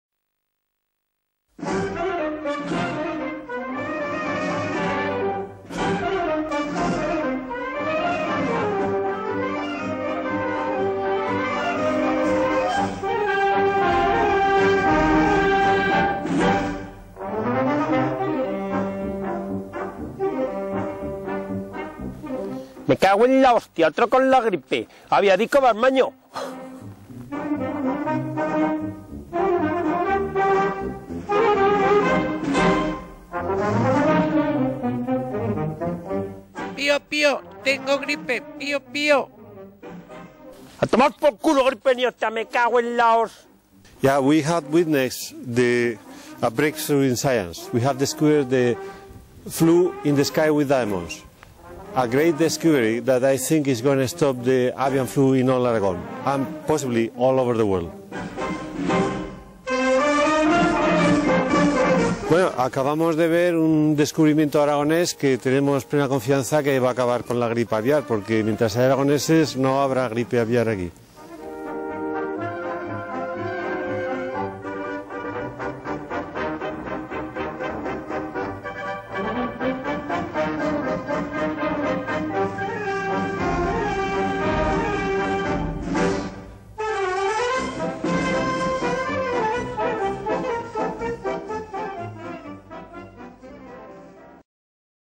Otre gripe
Primer corto que se rodó bajo las premisas del Movimiento “Otri” y fue realizado tras la aparición de la gripe aviar y rodado en Osia, (Jaca), el 26 de febrero de 2006, protagonizado por Paco Simón y Jesús Sáinz y dirigido por Dionisio Sánchez